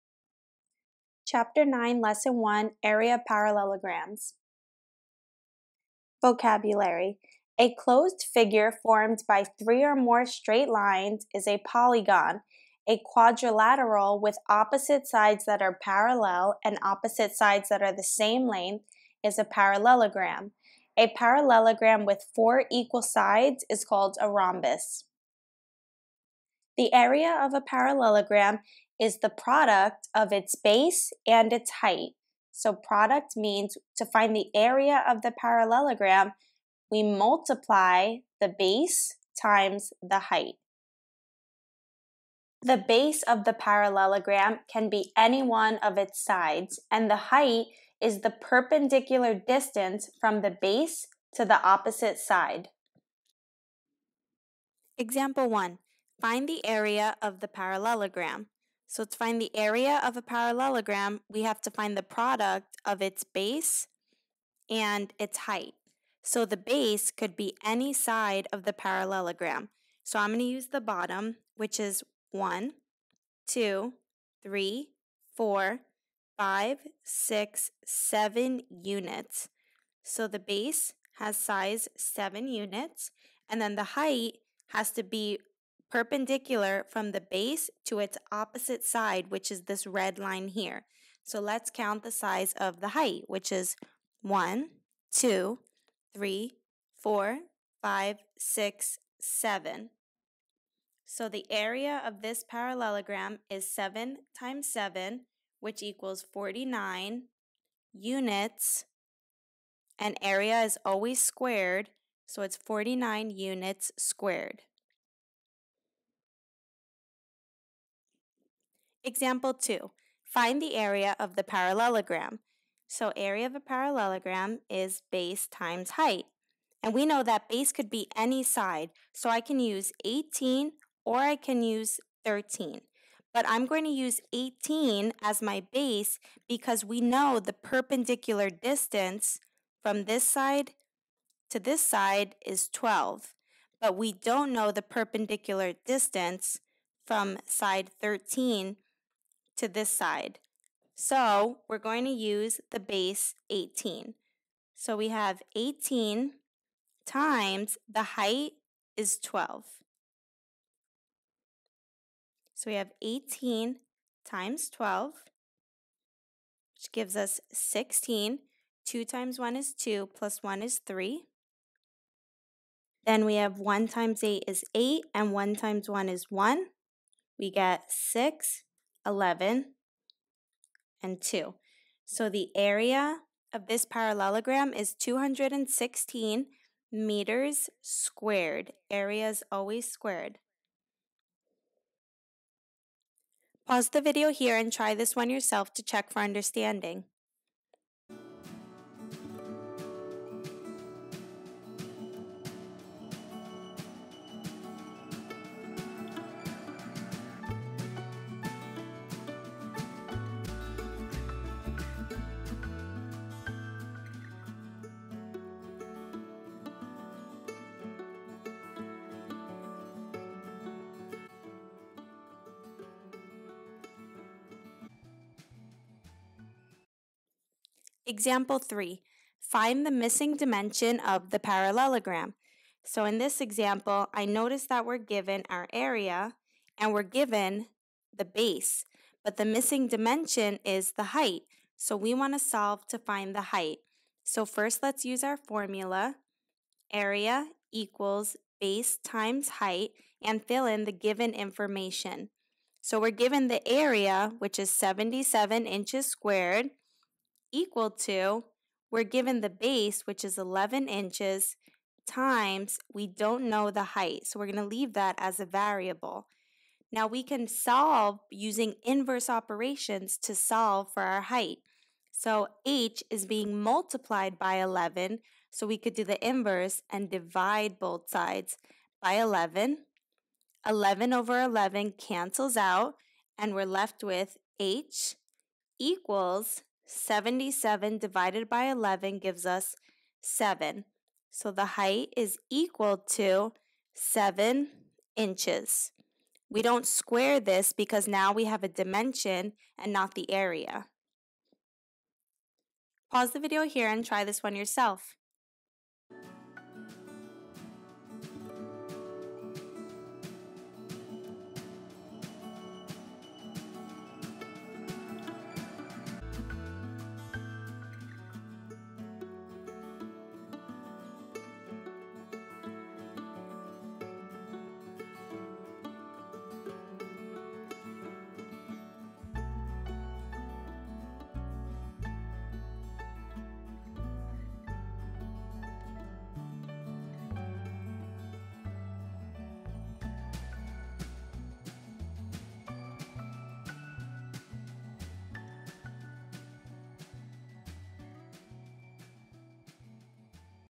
Chapter 9, Lesson 1 - Area of Parallelograms
This is a 6th grade math tutorial video on finding the area of parallelograms.